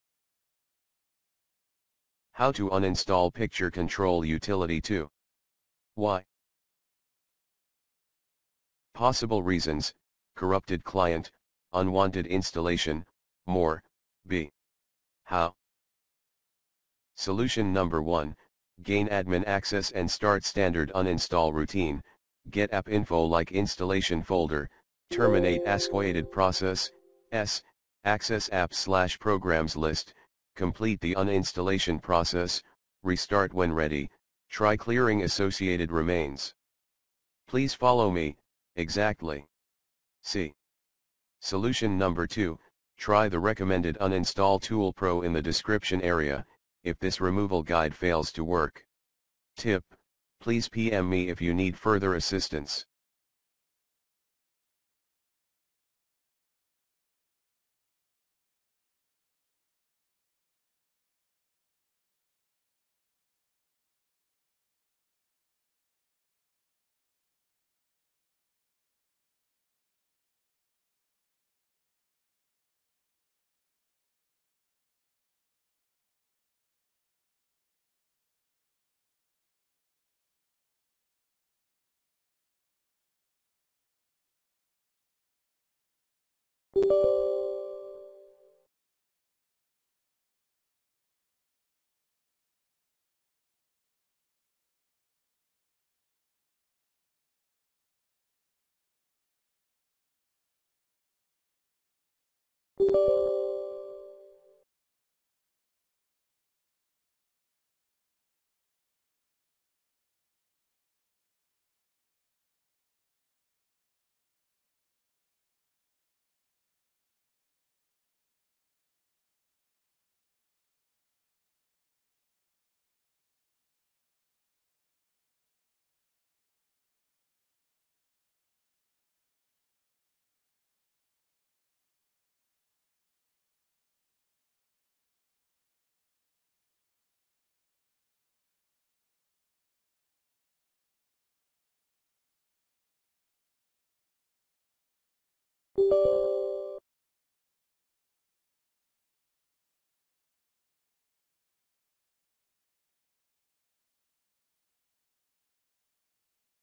Uninstall Picture Control Utility 2 on Windows 10
This vid explains how to uninstall Picture Control Utility manually. Plz use this uninstaller tool PRO@ if you are having trouble doing so.